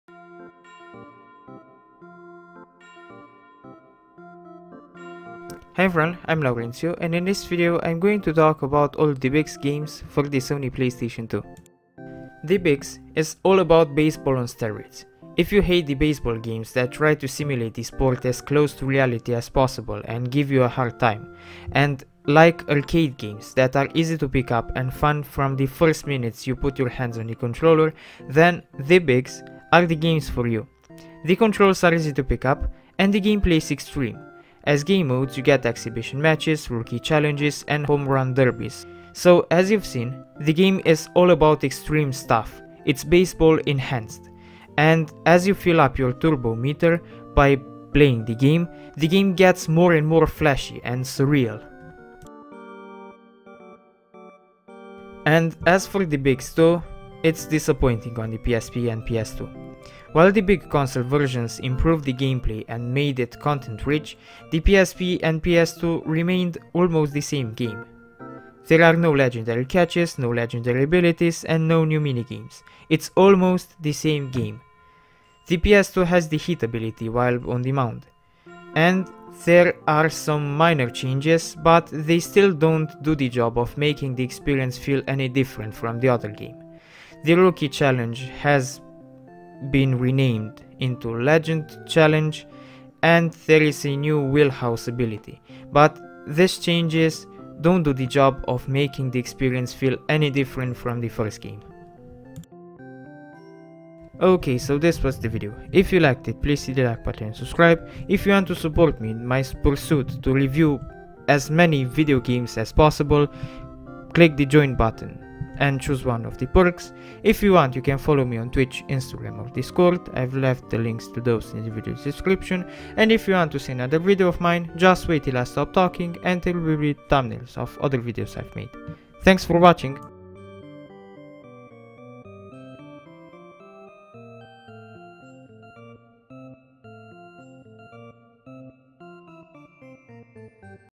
All The Bigs Games for PS2 Review
Here is a list of all The Bigs games for the SONY Playstation 2:
1. The Bigs
2. The Bigs 2